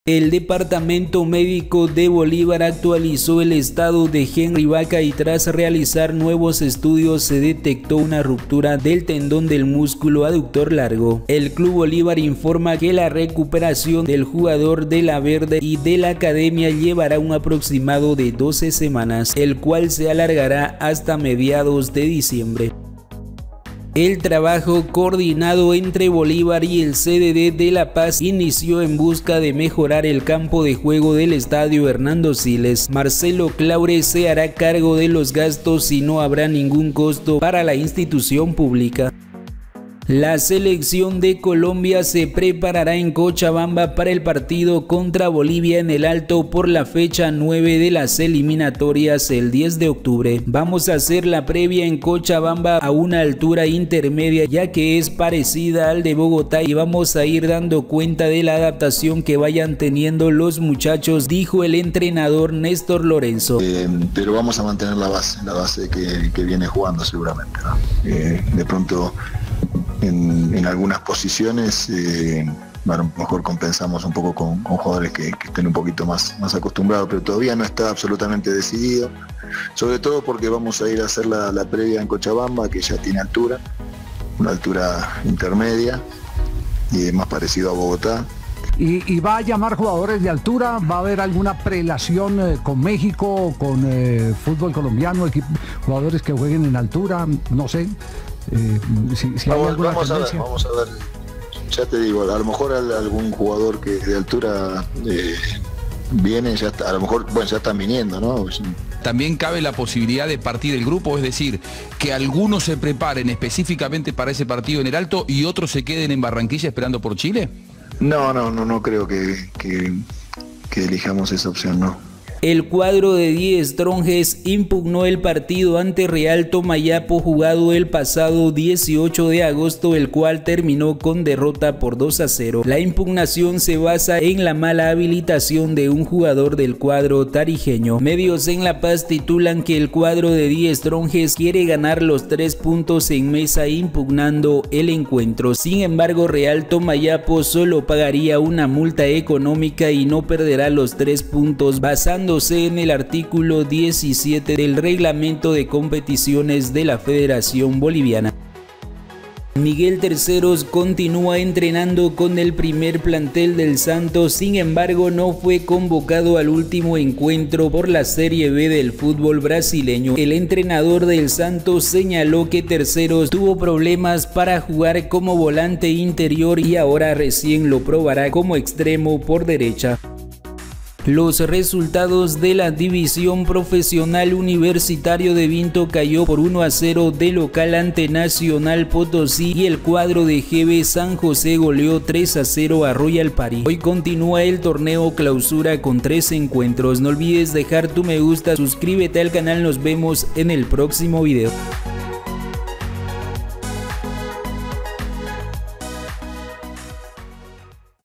NUEVA POSICION DE MIGUEL TERCEROS | SE ACABÓ LA TEMPORADA PARA HENRY |COLOMBIA NO ARMARÁ DOS EQUIPOS
Noticias Sobre Futbol, Torneo Apertura, Torneo Clausura, Futbol Boliviano e Internacional, Copa Libertadores y Sudamericana.